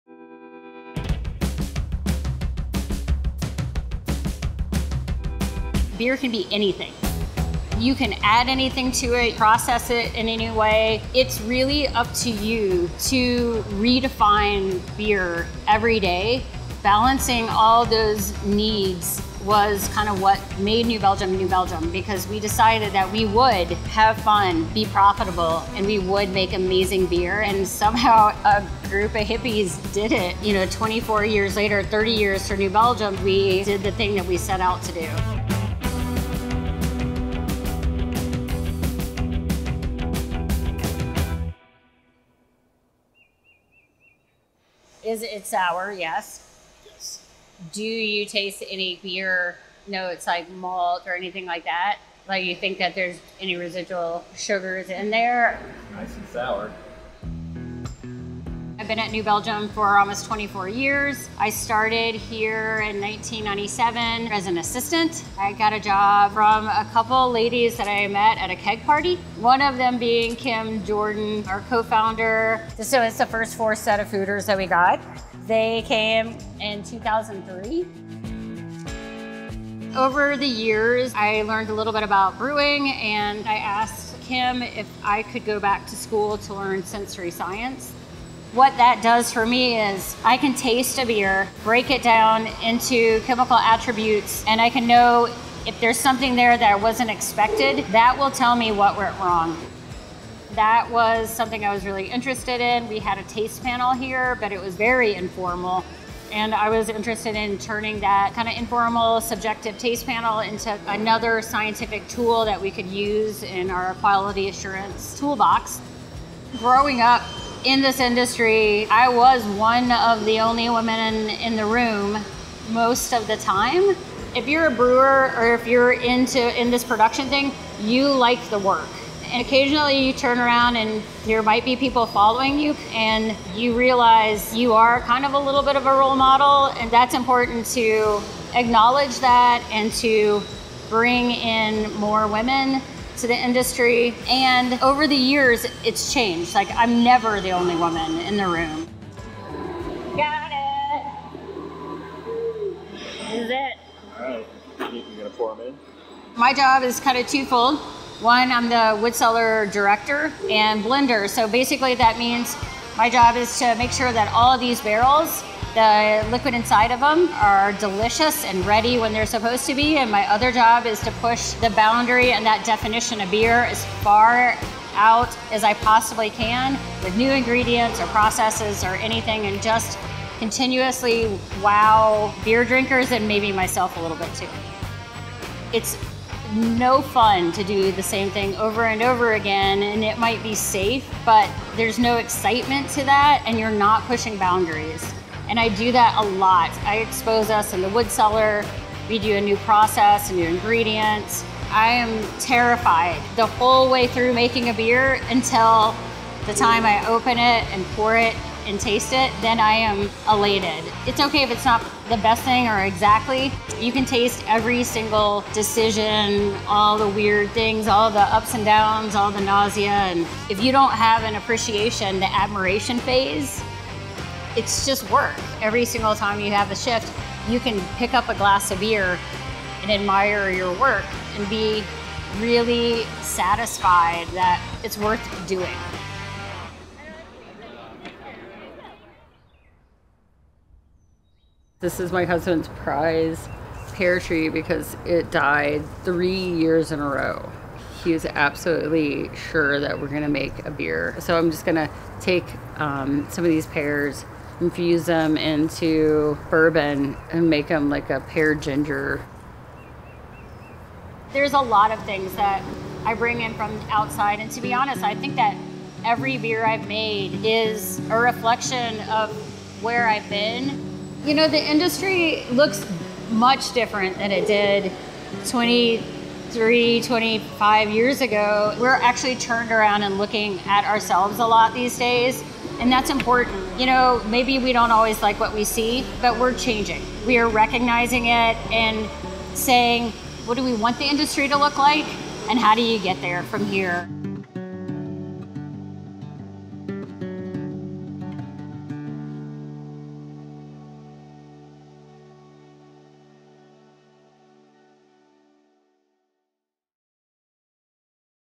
How New Belgium Brewing Makes Some of Your Favorite Beers | Made Here | Popular Mechanics
The company's ethos? “Have fun, be profitable, and make amazing beer.”

Fort Collins, Colorado has a lot to offer: sprawling scenic views, historical sites, and the New Belgium Brewing Company.

“Beer can be anything,” says Lauren Woods Limbach, New Belgium’s wood cellar director and blender. Limbach, who has been with the brewery since 1997, loves the thrill of working on a new batch because there’s room for creativity throughout the entire process. “It’s really up to you to redefine beer everyday,” Limbach tells Popular Mechanics.